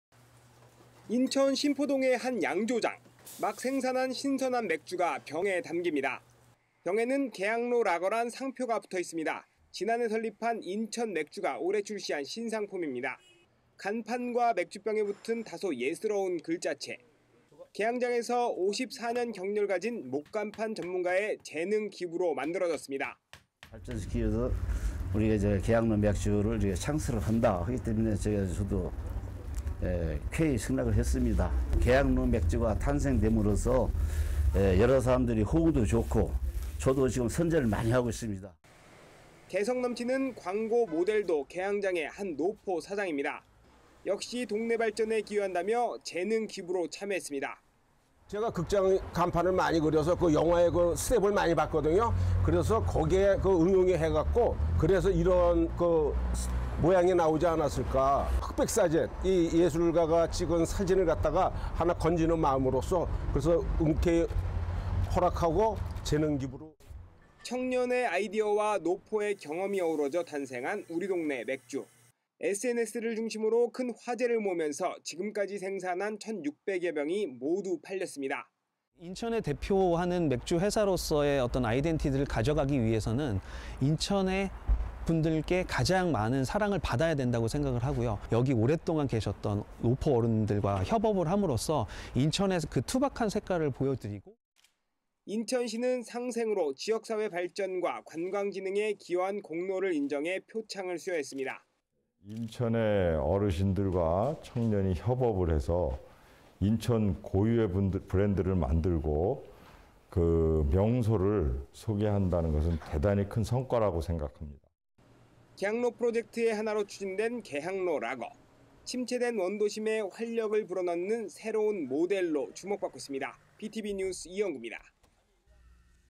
[인천]노포와 청년의 합작품…향토 맥주 '인기'/SK브로드밴드 뉴스
인천 개항장의 노포와 지역 양조장이 힘을 모아 인천 맥주를 만들었습니다.
노포 상인들이 모델로 참여하고, 제품 글자체도 직접 만들었다고 하는데요.
선풍적인 인기를 끌면서 지금까지 생산한 제품이 모두 팔렸다고 합니다.